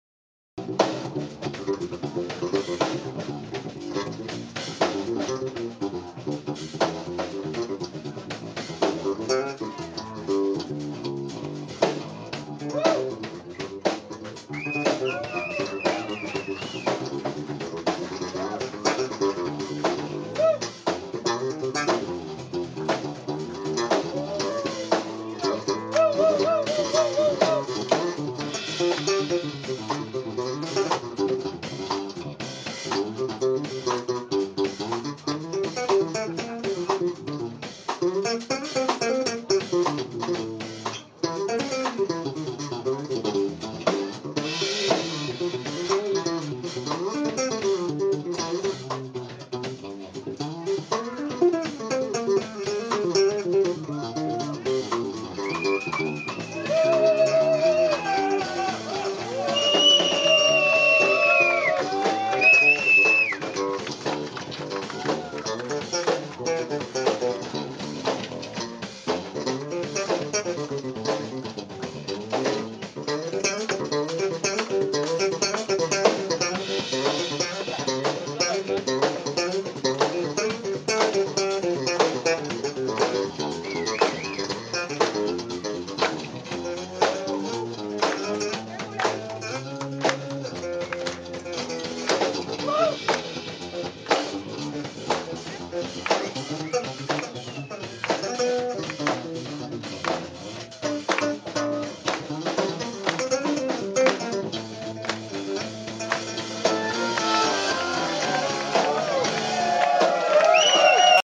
Ana Popovic@Hot Jazz Club Münster - Bass-solo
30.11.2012. Hot Jazz Club Münster. Ana Popovic und Band. Hier ein Teil des großartigen Solos von Ronald Jonker